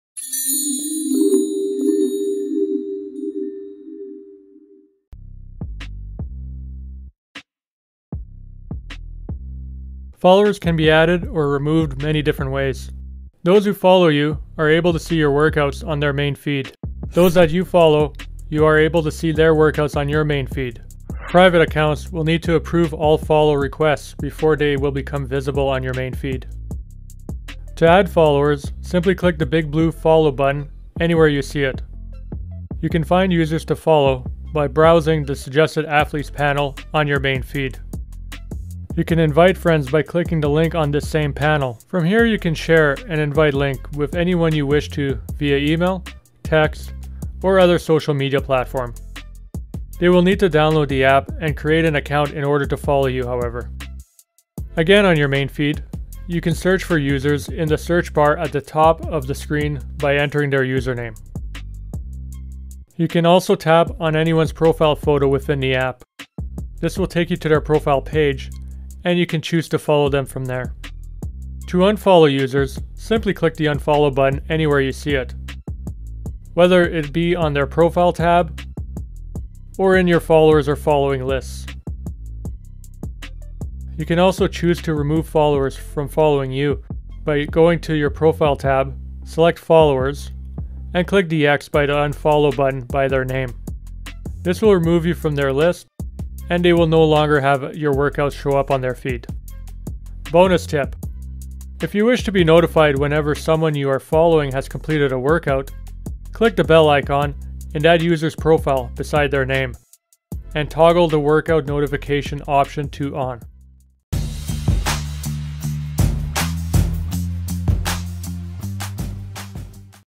Hevy Workout Tracker | How do I manage followers?
Welcome to my Hevy Workout App Tutorial series.  The series is intended to answer questions as well as show users how to use the Hevy Workout Tracker and its many features!

In this video we review how to manage followers, where to find them, and how to remove them.  By the end of this video, Hevy Workout Tracker | How do I manage followers, you will know exactly how to do just that!